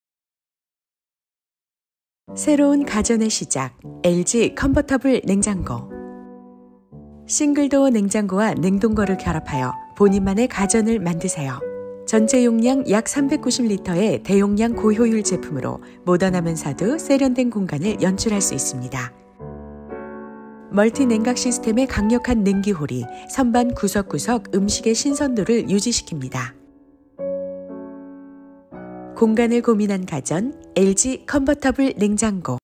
20211117 LG Convertible Refrigerator / Black Friday Sale / Product ad
This convertible refrigerator has two sections or compartments separately. It was a groundbreaking idea product of LG. However, there was no advertisement video for overseas export products. So, this video was specially created by me without any other original video.

*Source of product : LG KOREA's Marketing team